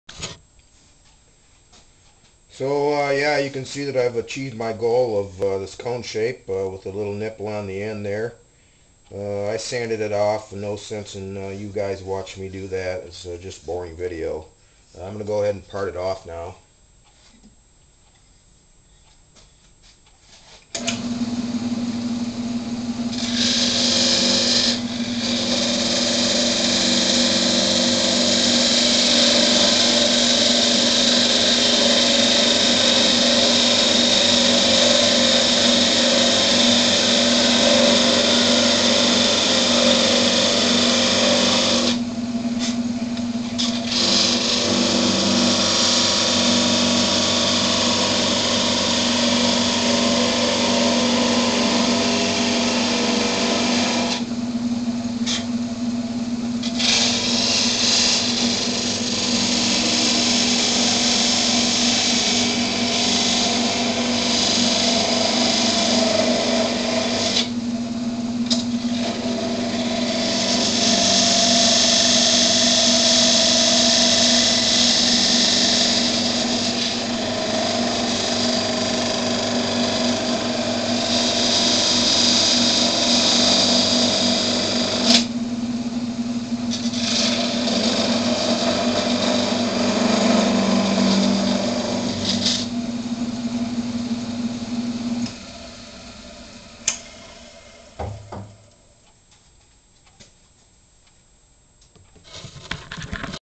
Wood lathe project-Spinning top pt. 2
A quick demonstration on how I turn an old fashion spinning top